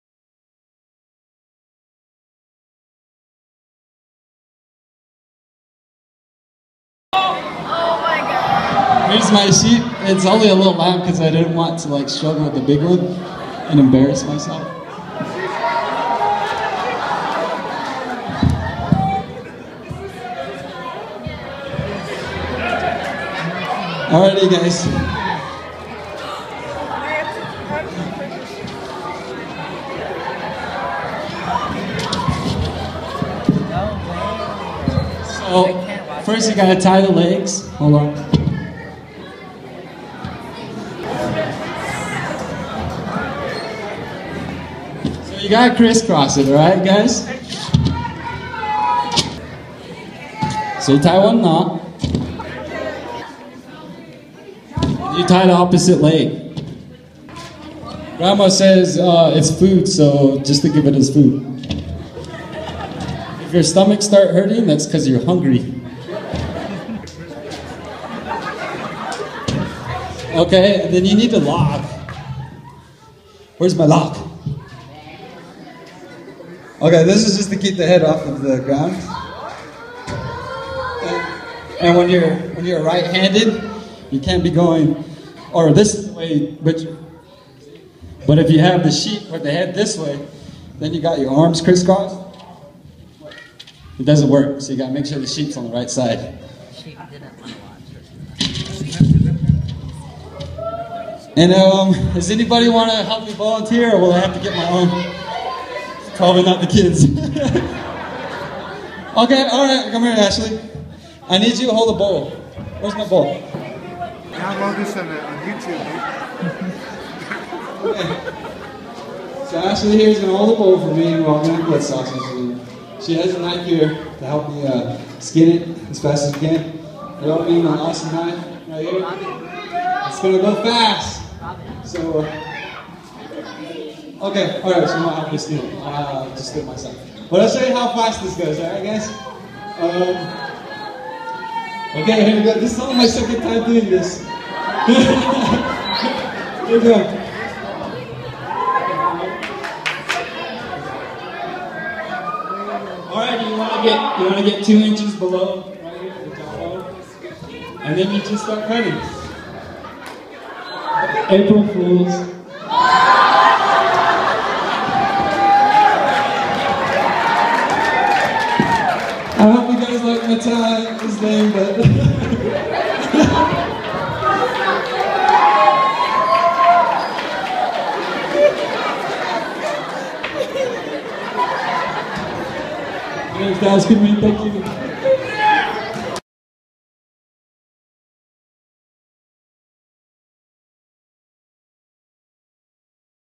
Best April Fools at Dine College!!!
Navajo student butchering sheep.